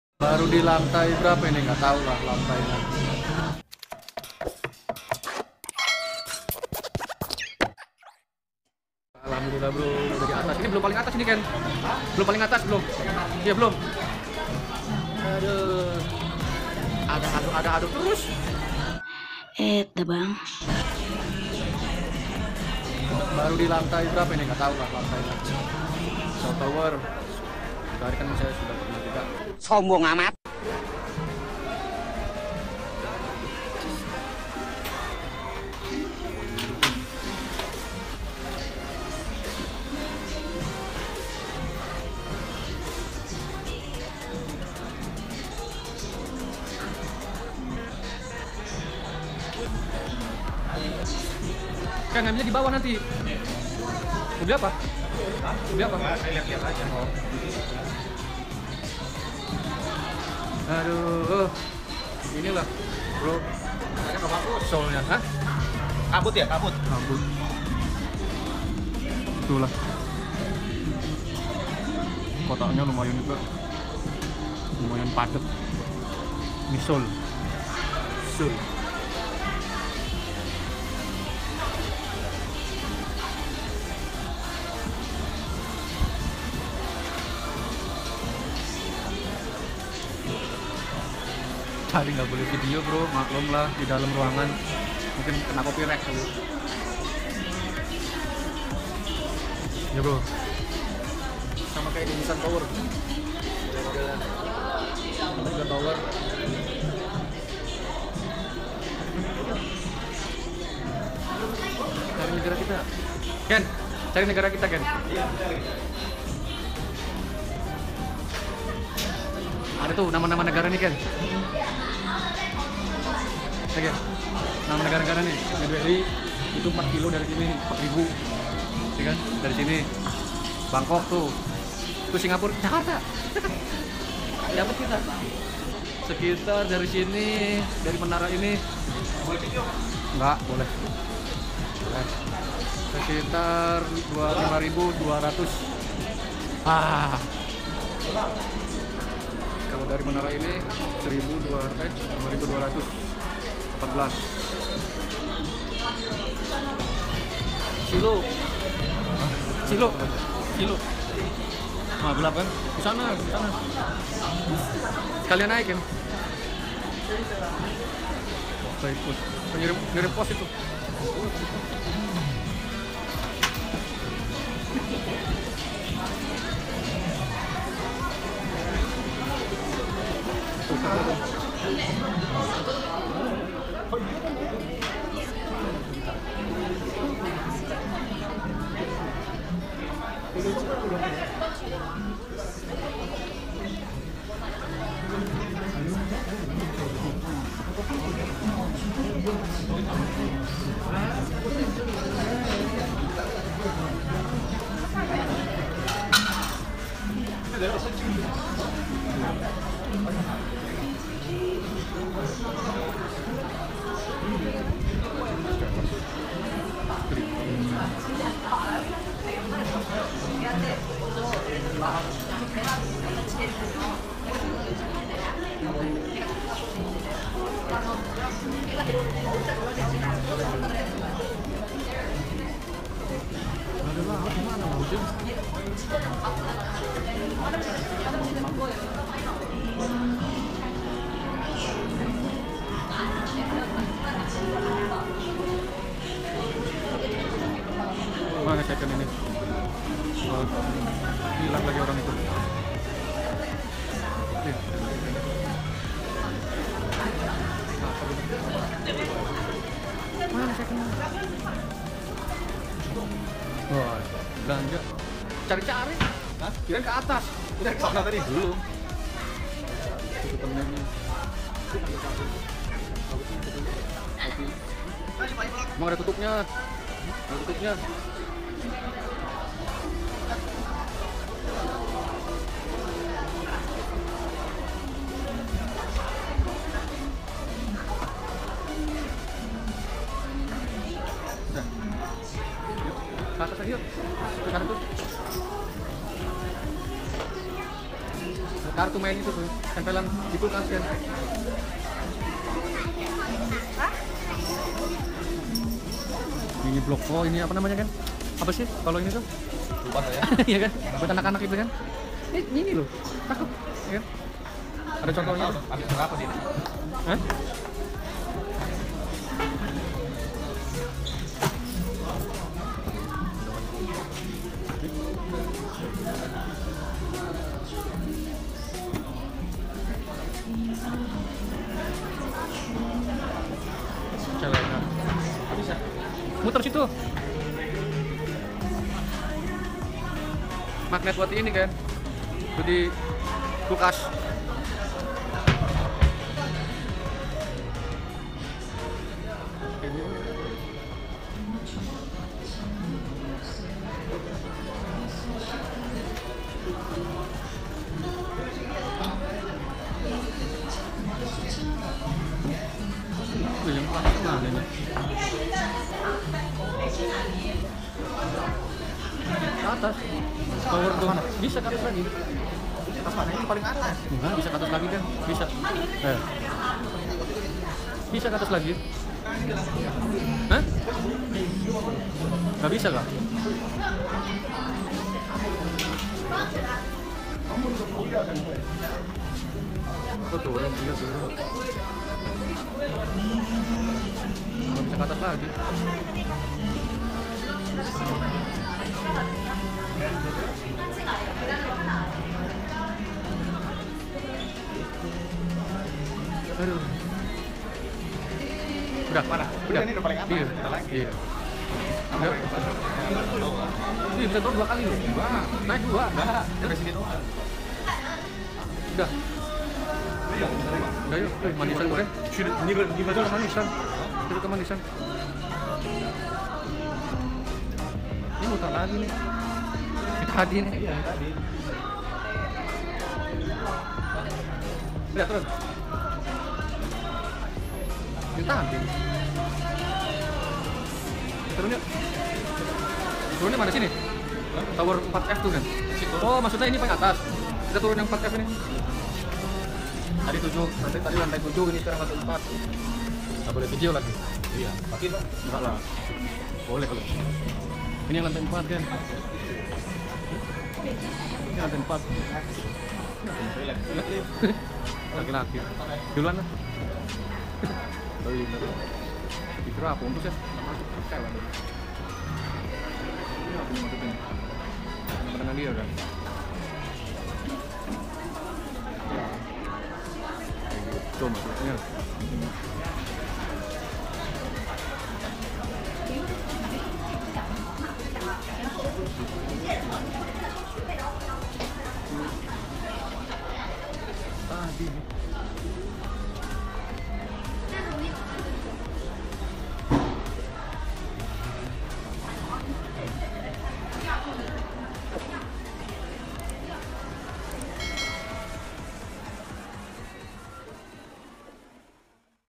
Melihat Kota Seoul Dari Atas Seoul Tower
Keindahan kota seoul dilihat dari atas tower seoul